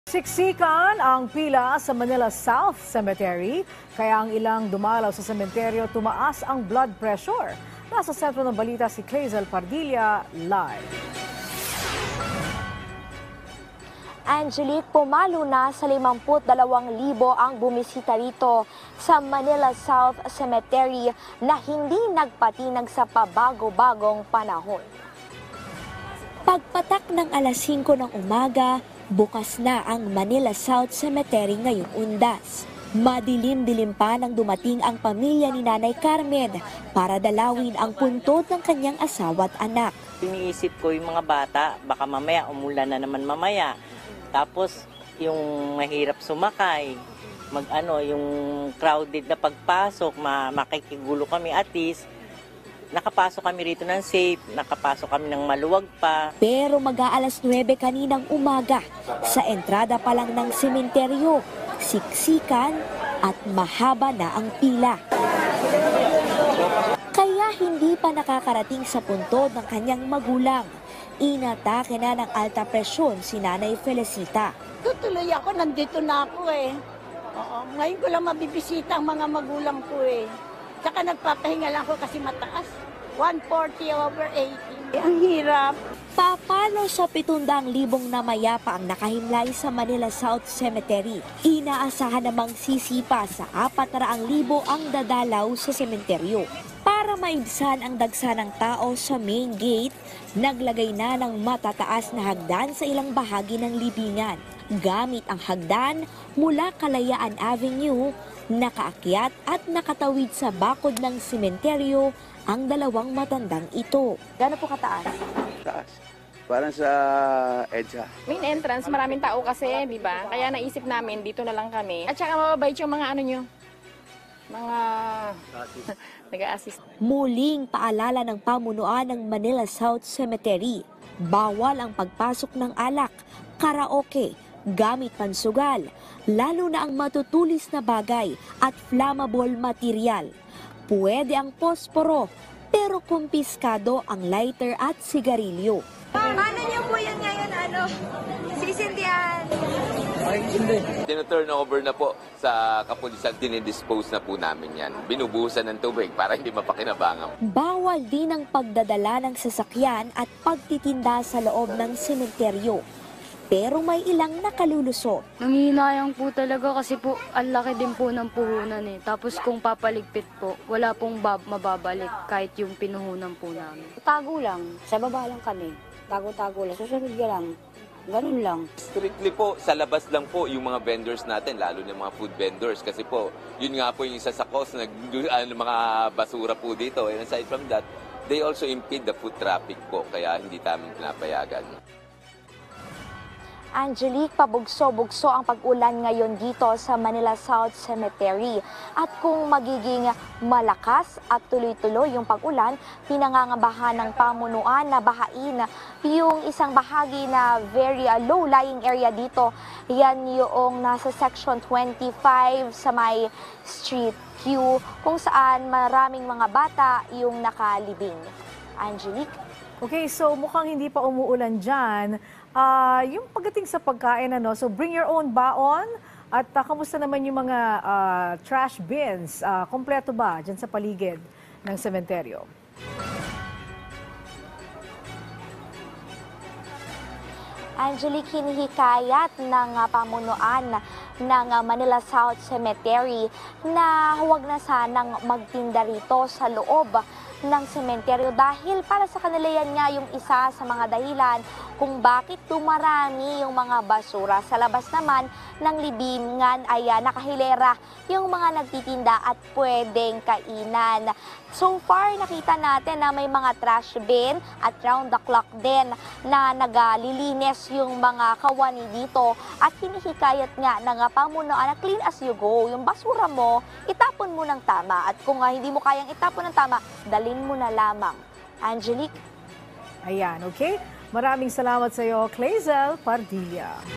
Manila South Cemetery, maagang dinagsa;
Mga hagdan, inilagay sa bakod ng sementeryo para mabawasan ang dagsa sa entrada nito